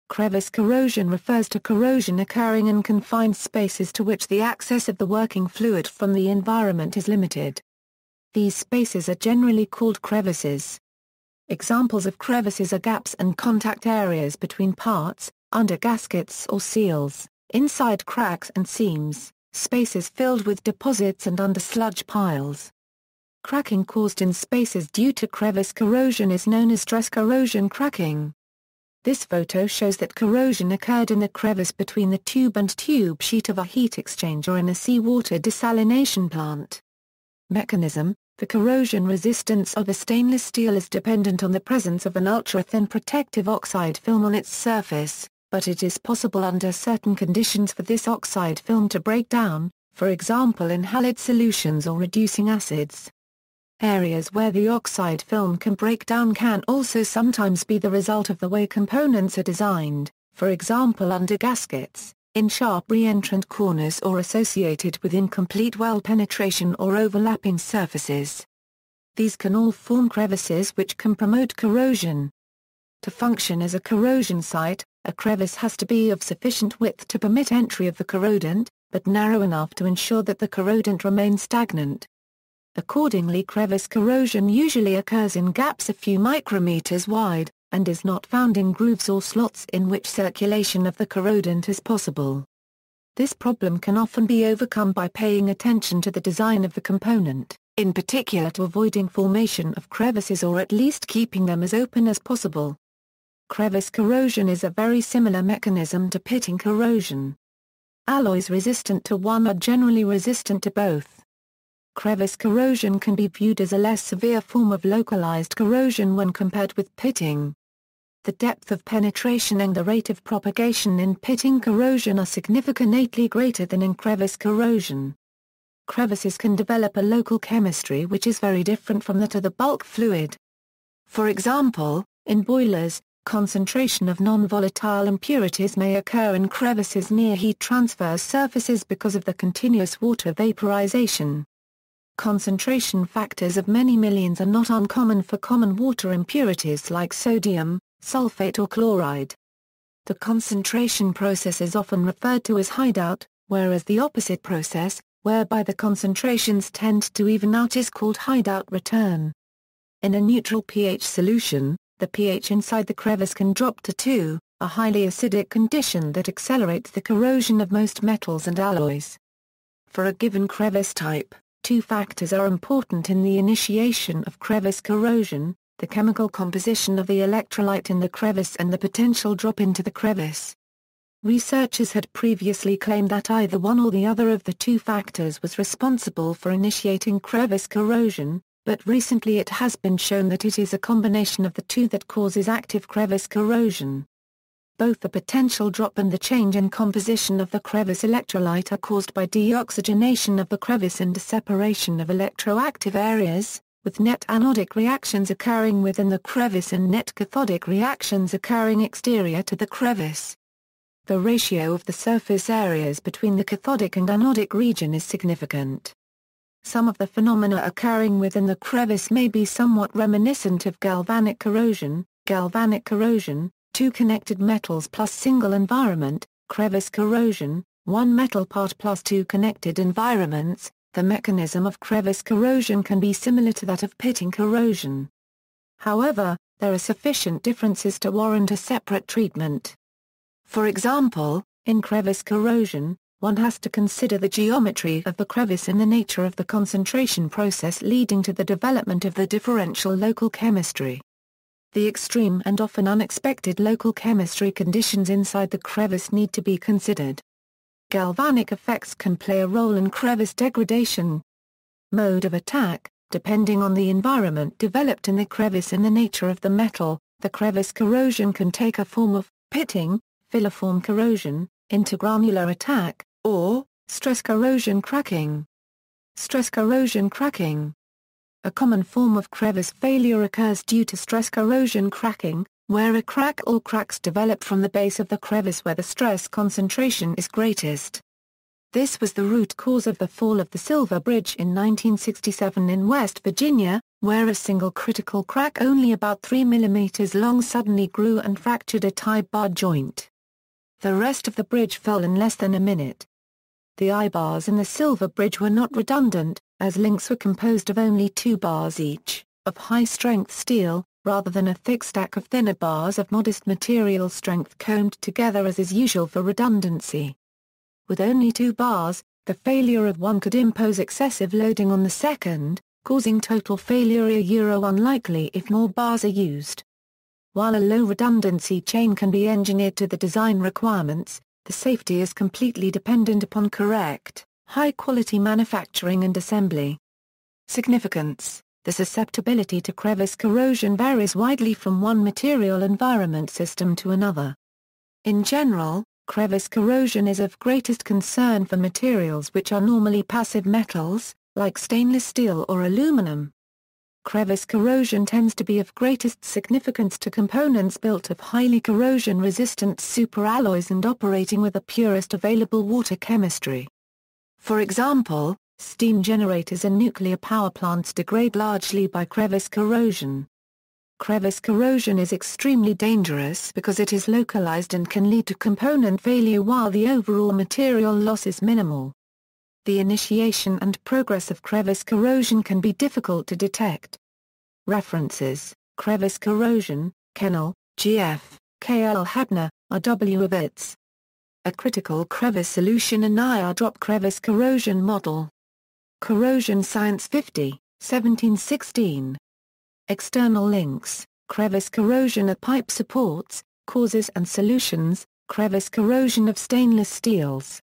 Crevice corrosion
Crevice corrosion refers to corrosion occurring in confined spaces to which the access of the working fluid from the environment is limited. These spaces are generally called crevices. Examples of crevices are gaps and contact areas between parts, under gaskets or seals, inside cracks and seams, spaces filled with deposits and under sludge piles. Cracking caused in spaces due to crevice corrosion is known as 'stress corrosion cracking'.
This photo shows that corrosion occurred in the crevice between the tube and tube sheet of a heat exchanger in a sea water desalination plant.